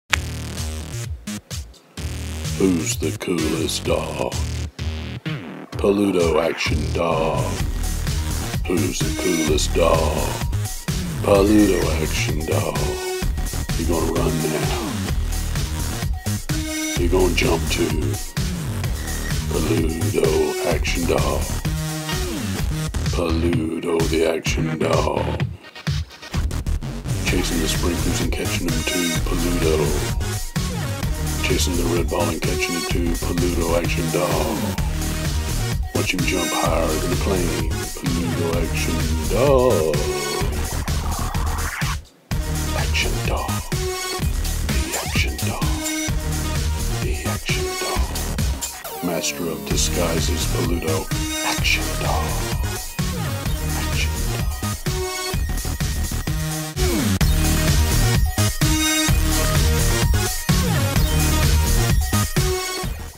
PELUDO ACTION DOG (The Hairless ChiWeiner Dog)
Dances with Sheep brings you the newest Super Hero!  The Dog of action, PELUDO!!